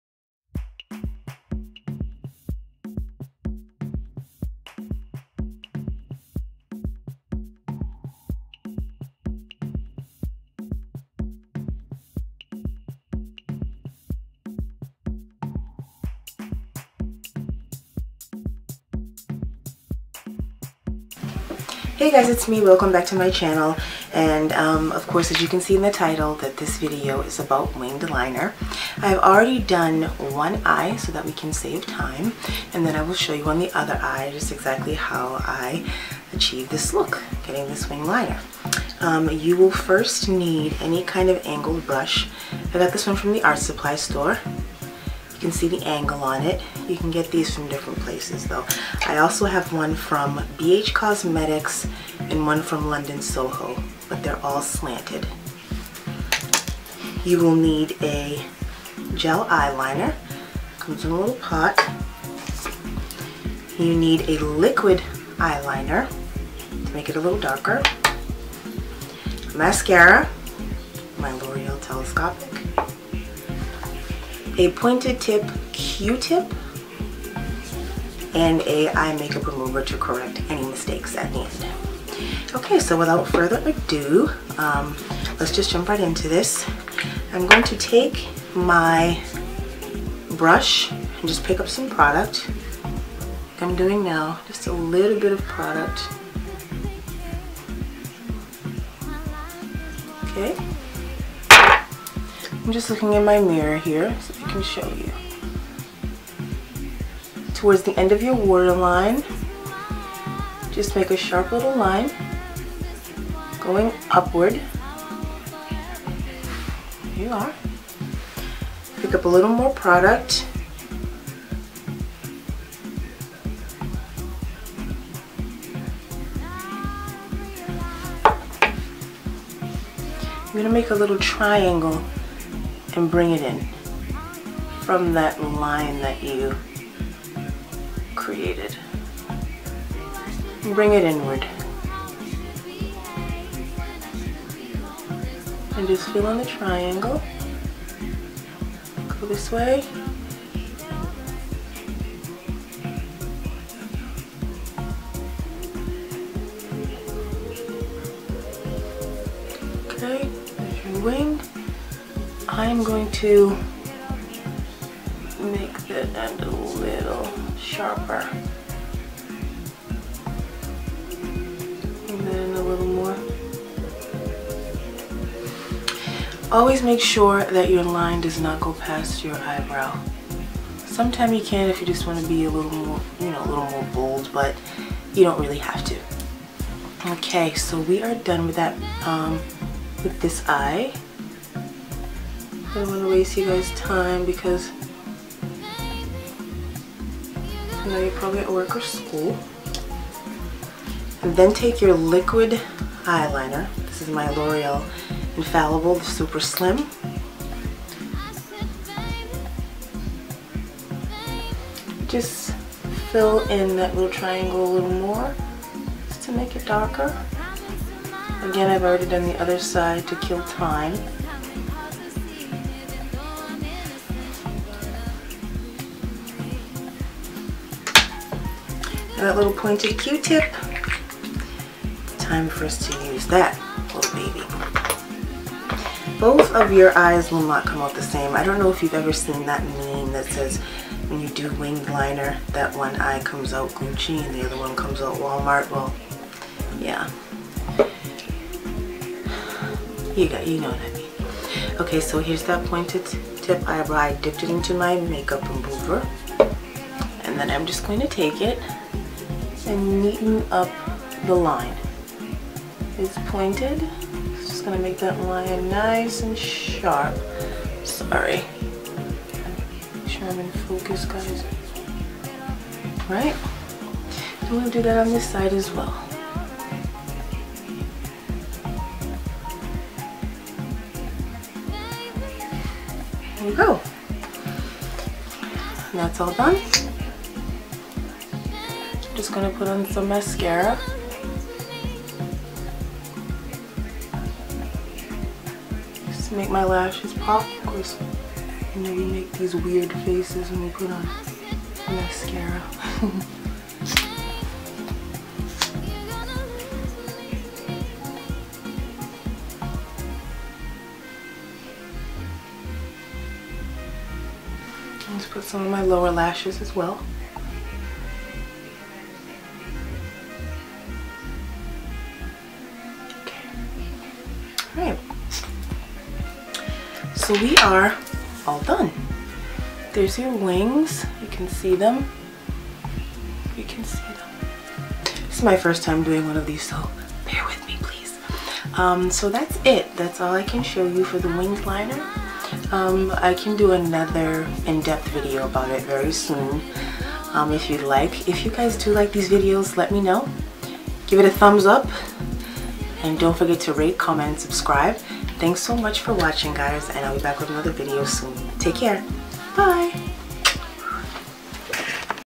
Beauty Basics 101: Winged Liner
My First Tutorial is about what else... Winged Liner! I hope you guys like it! I just started so bear with me my Loves!

Products:
Estee Lauder DoubleWear Foundation
Rimmel Stay Matte Powder
Mac Blunt Blush
Elf Baked Eyeshadow (enchanted)
Mac Lipliner (Brick)
Mac Lipstick (Ruby Woo)
Anastasia Dipbrow Pomade (Auburn)
Loreal Telescopic Mascara
Loreal infallible (the super slim) liquid Liner
Bobbi Brown Gel Eyeliner ( Black Ink)
Art Supply Brush (Angular Shader) 1/4